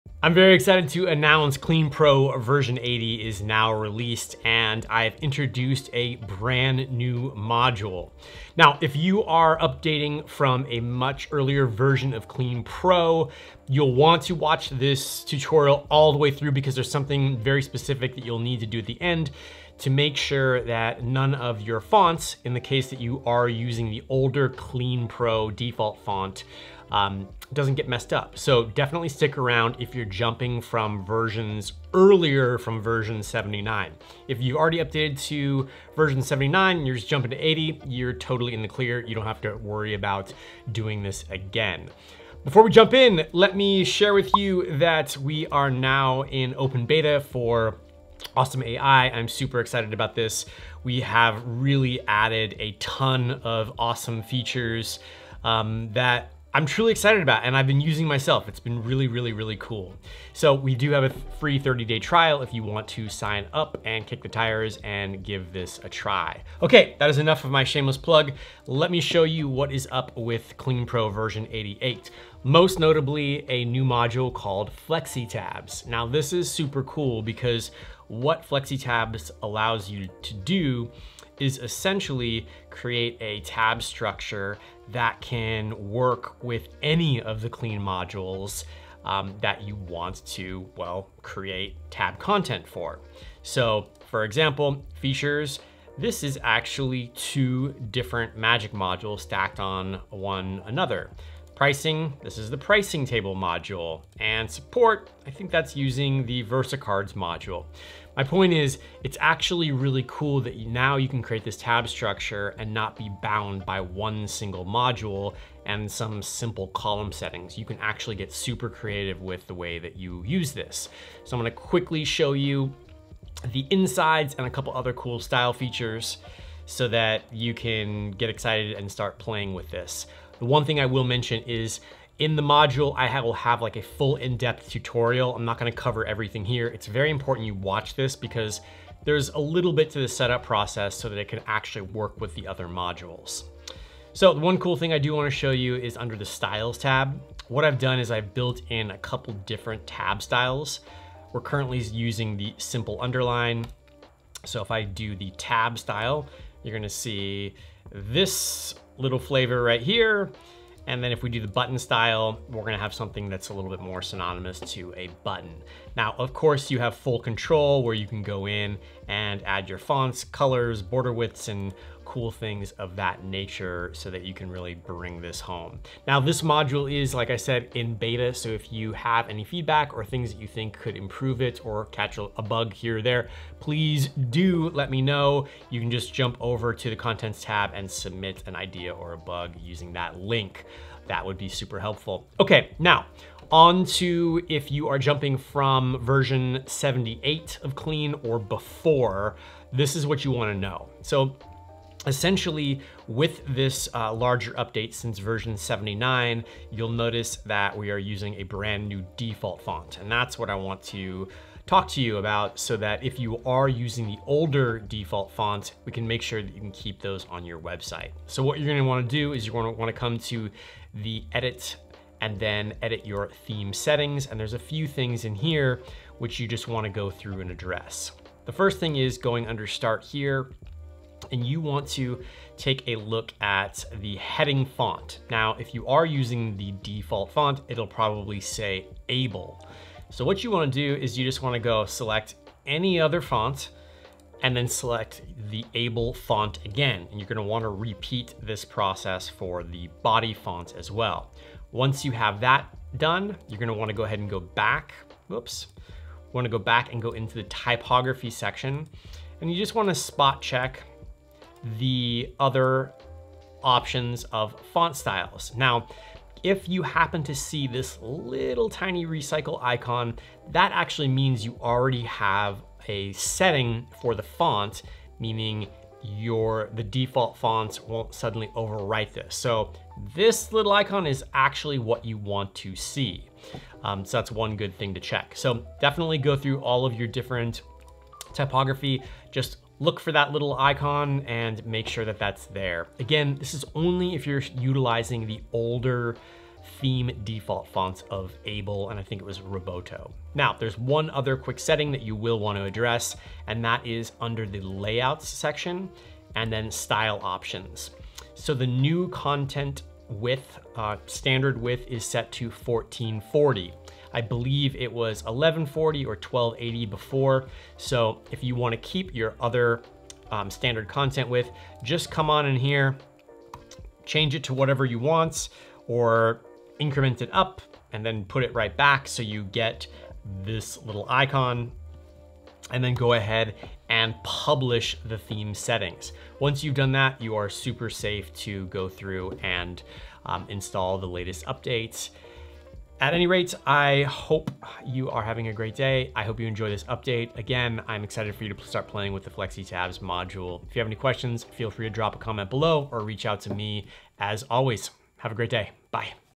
WATCH FIRST! Clean Pro v80 for the HubSpot CMS.
IMPORTANT! If you are currently on Clean Pro v78 or prior watch this video before updating as there are some important steps. If you are currently on v79 you're all good.